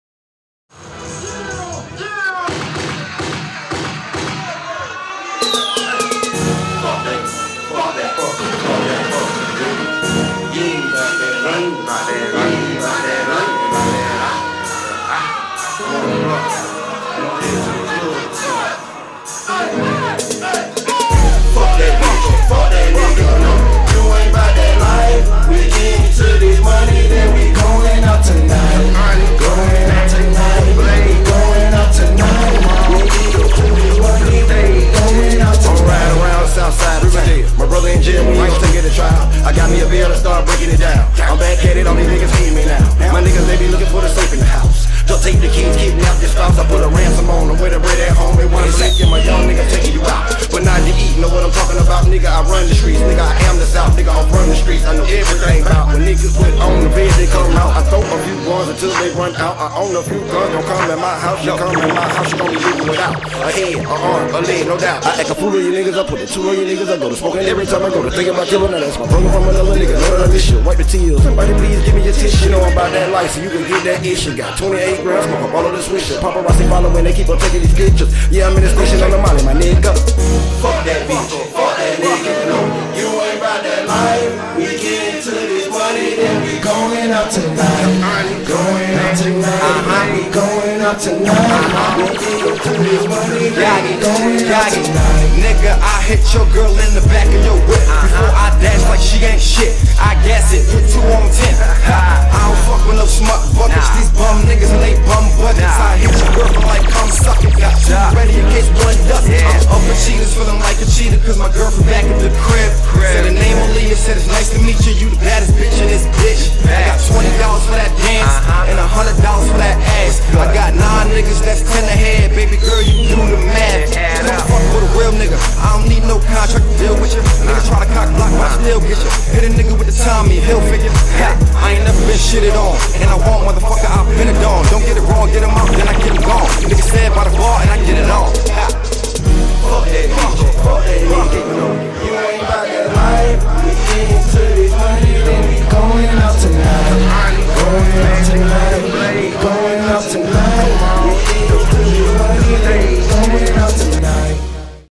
KORRI BETHEL-PERFORMING LIVE AT J PAUL'S REMIXED VIDEO VERSION
KORRI BETHEL PERFORMING LIVE AT J PAUL'S IN RIVERDALE. REMIXED VERSION